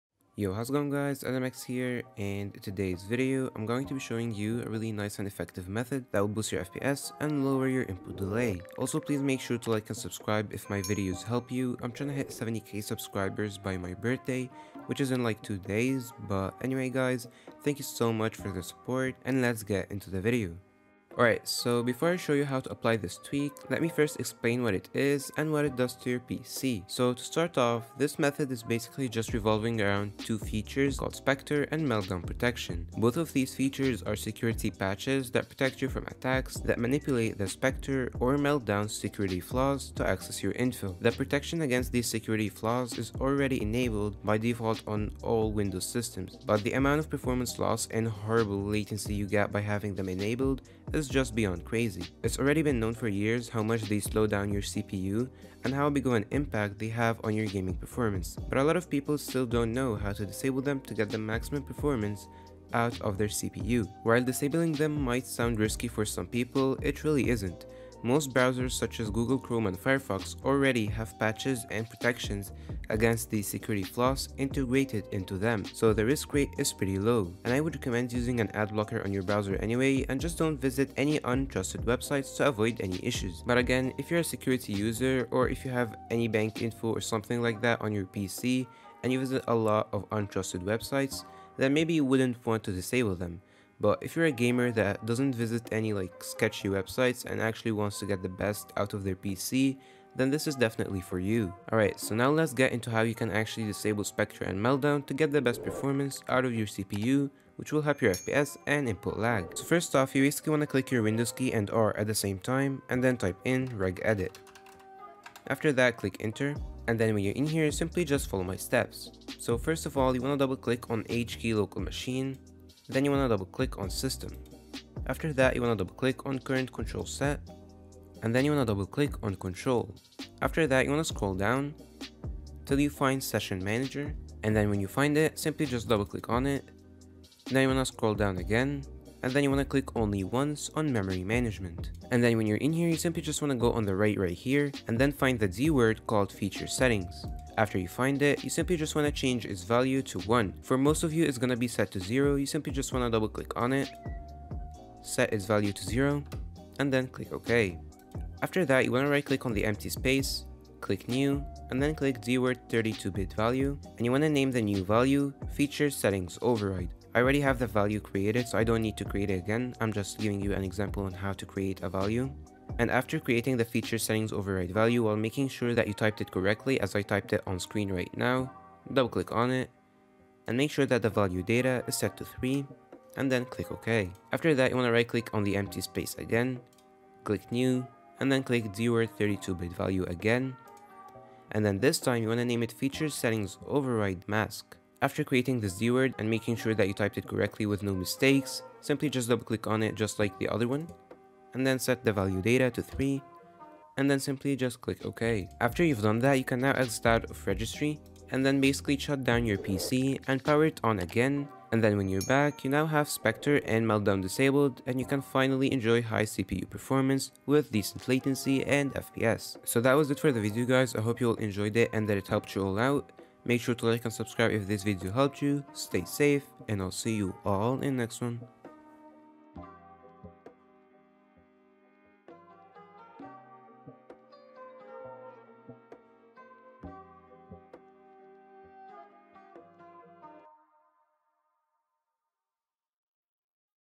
Higher FPS & Reduce Input Delay With This NEW Tweak! (Disabling Spectre & Meltdown Mitigations)
This video will guide you on how you can disable spectre and meltdown mitigations to get the maximum performance out of your CPU to increase performance (boost fps) and reduce input delay (input lag), and should you as a user even do it?